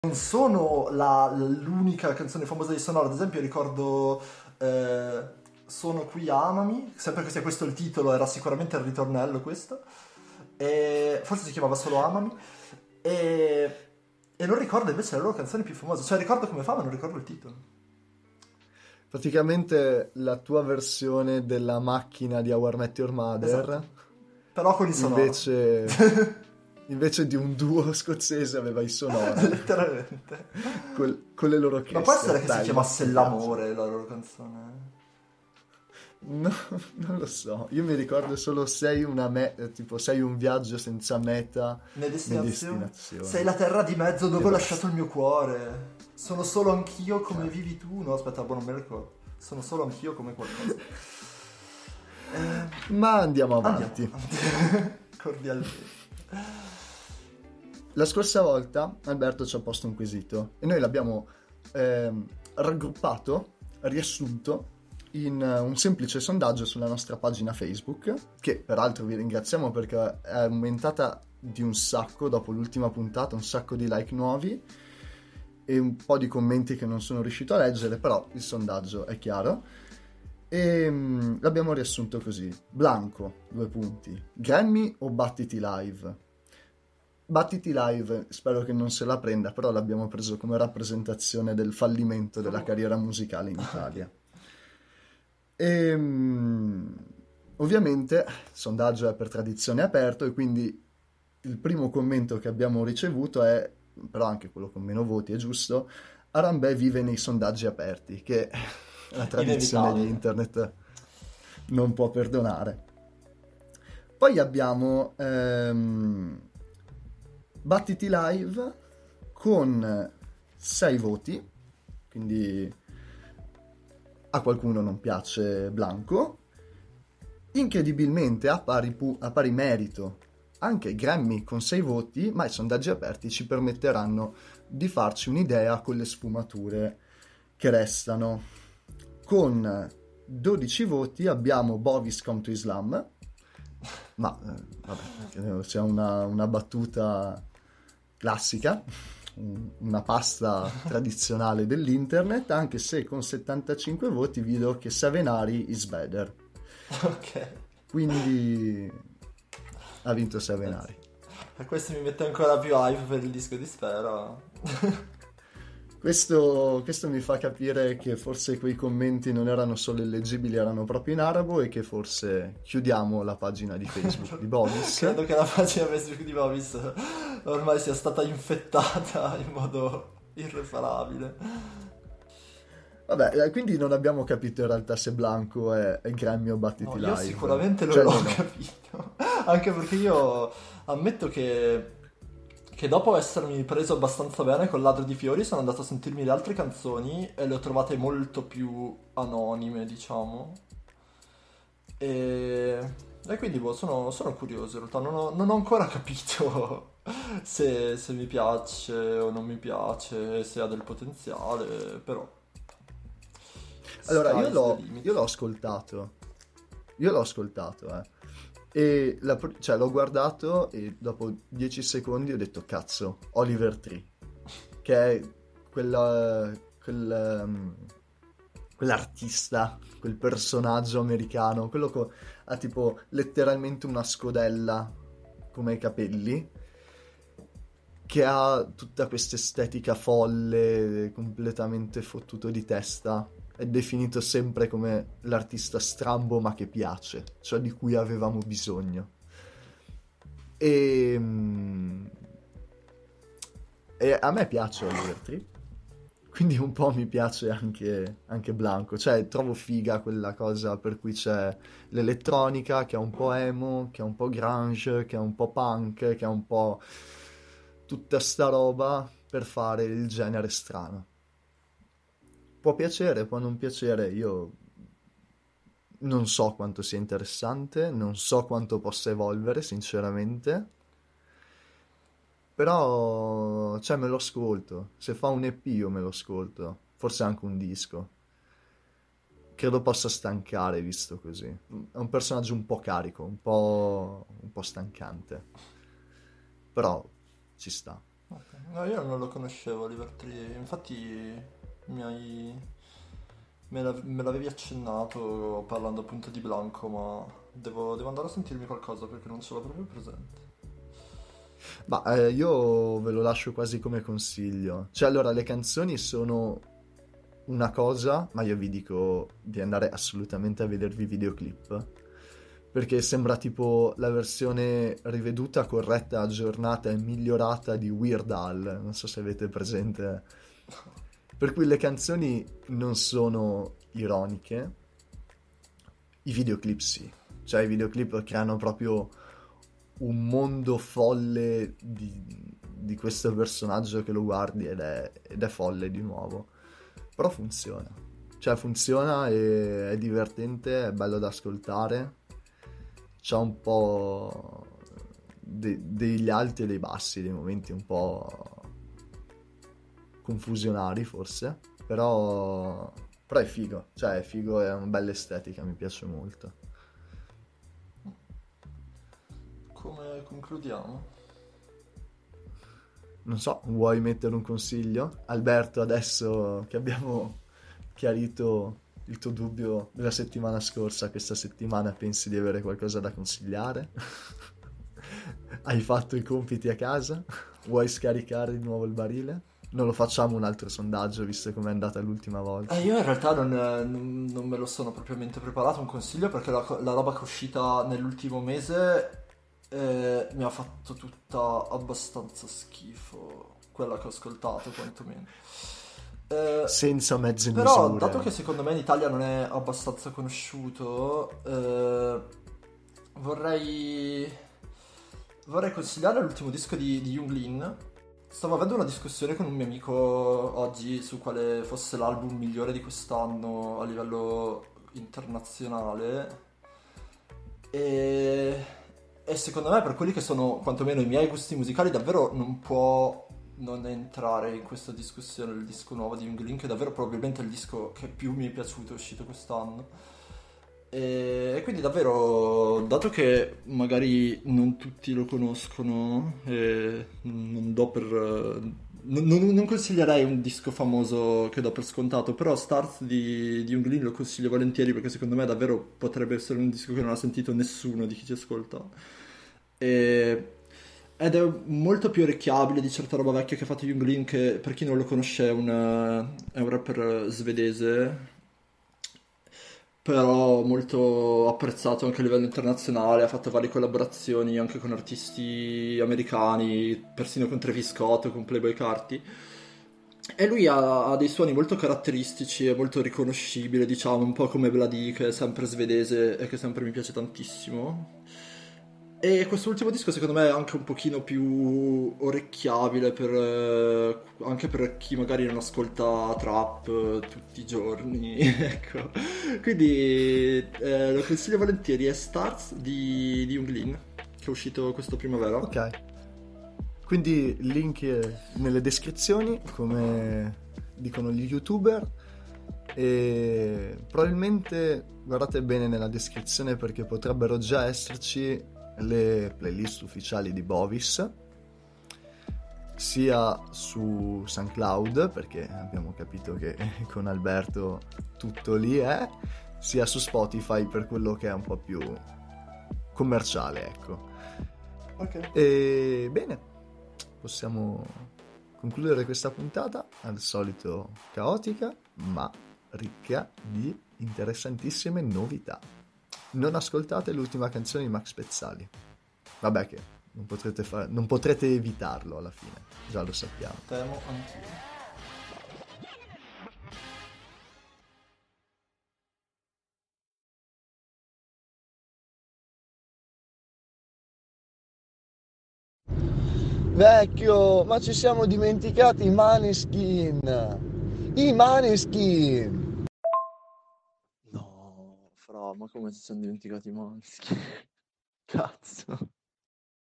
OHKO _ Bovis 005 (part 3 di 3)
In questa puntata si blatera di meteore, ritorni, il documentario famoso e ovviamente un po' di Anna